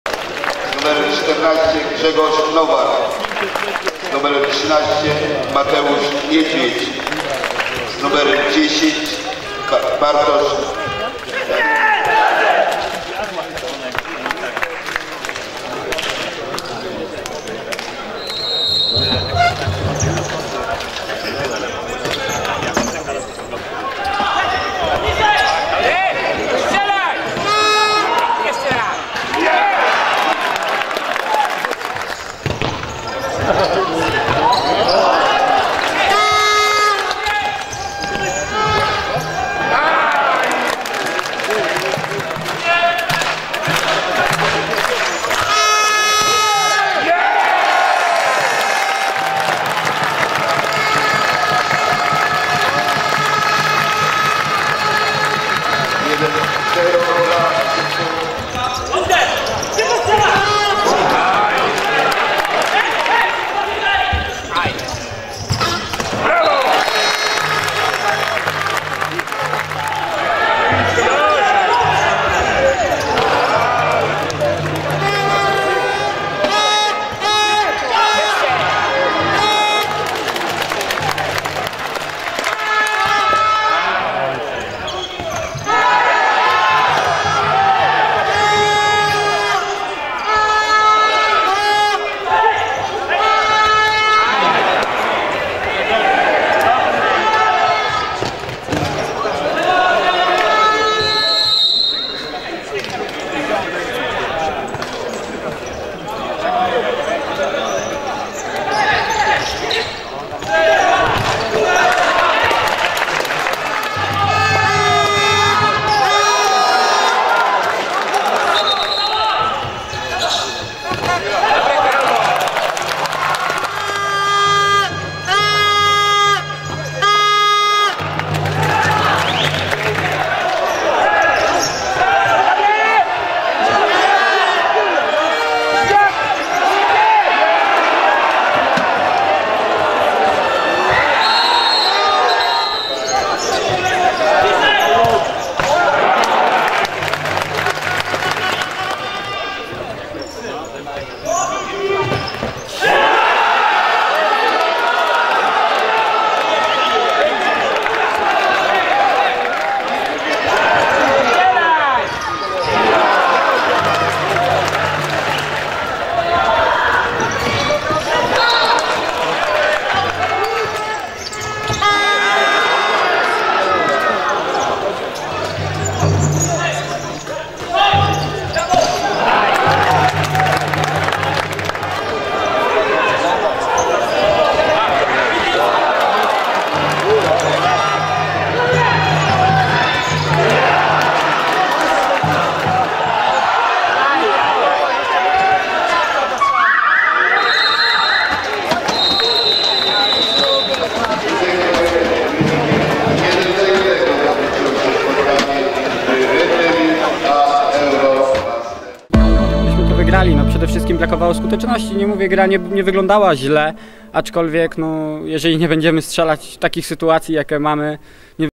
Red Devils Chojnice - KGHM Euromaster Chrobry Głogów / Skrót / Futsal Ekstraklasa [04.12.2015]
Oglądaj najciekawsze akcje z meczu Futsal Ekstraklasy: Red Devils Chojnice - KGHM Euromaster Chrobry Głogów z KP Sport